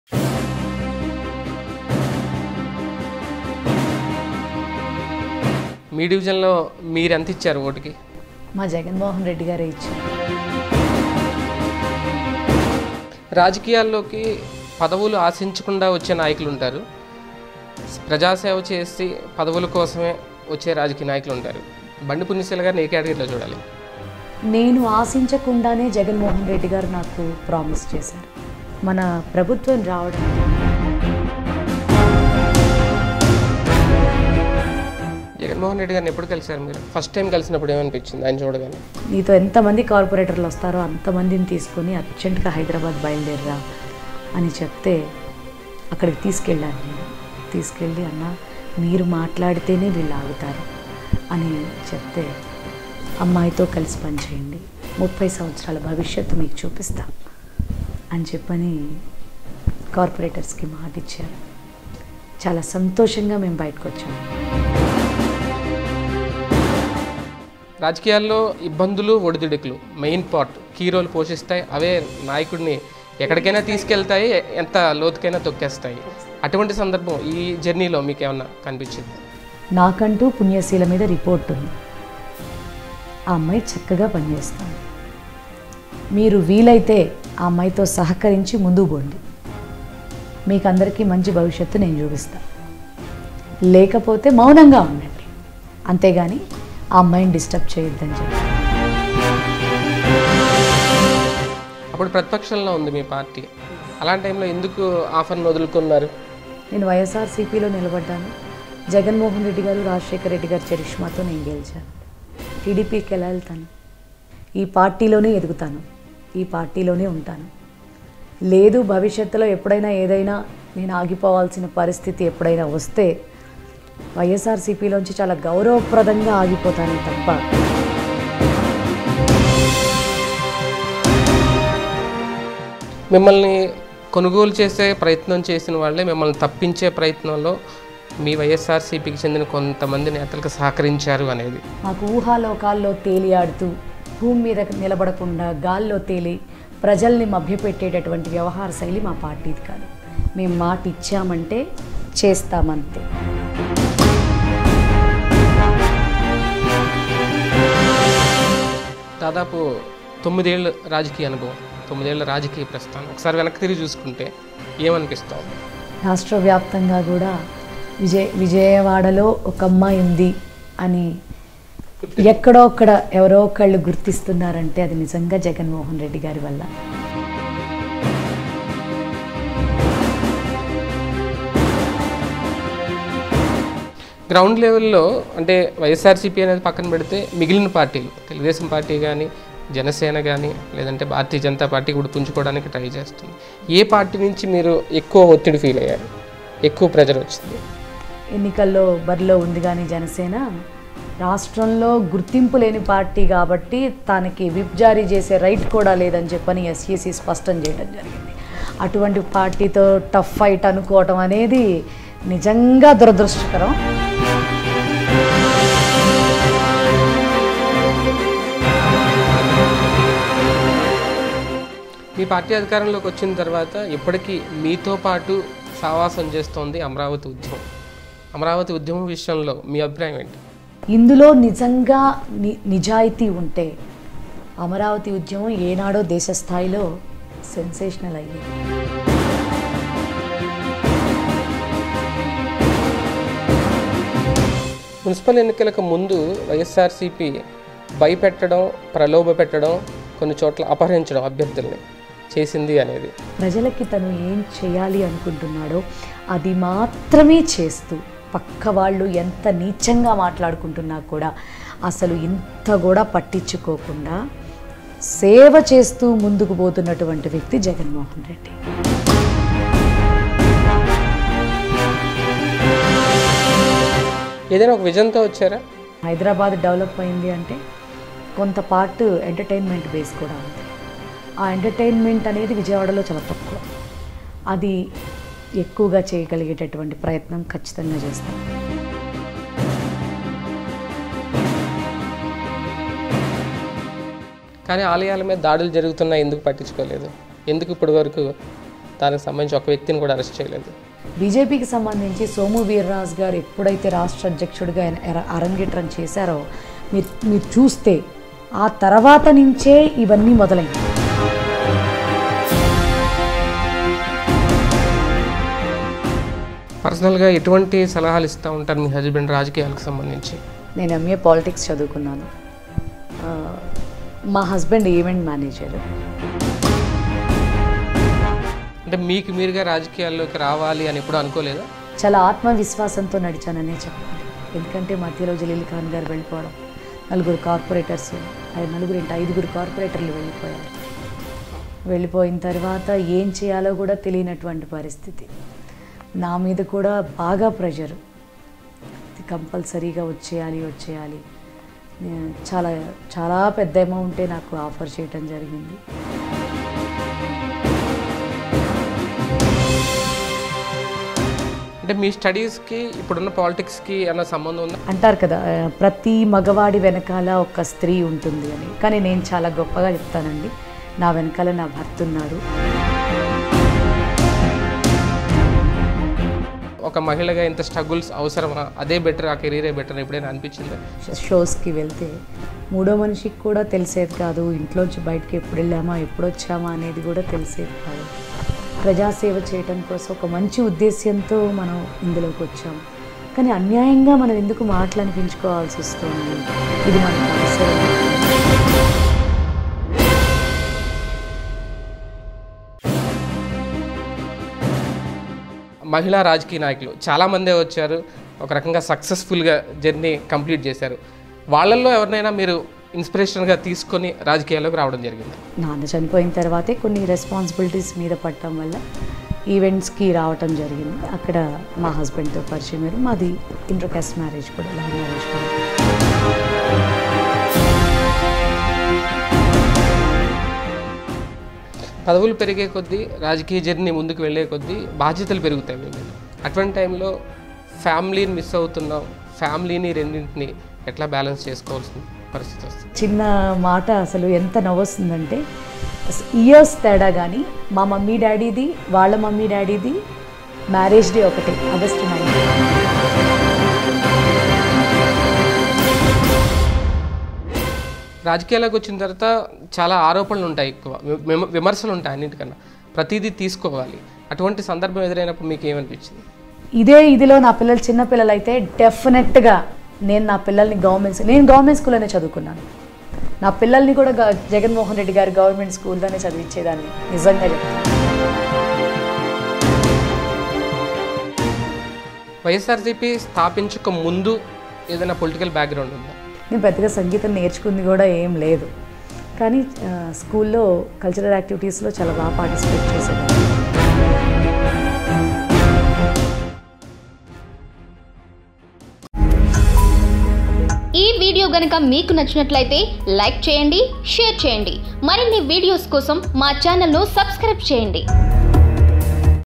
Bandi Punyaseela | Vijayawada Mayor Candidate | విజయవాడ మేయర్ రేసులో పుణ్యశీల | Ys Jagan |PDTV News
విజయవాడ మేయర్ రేసులో పుణ్యశీల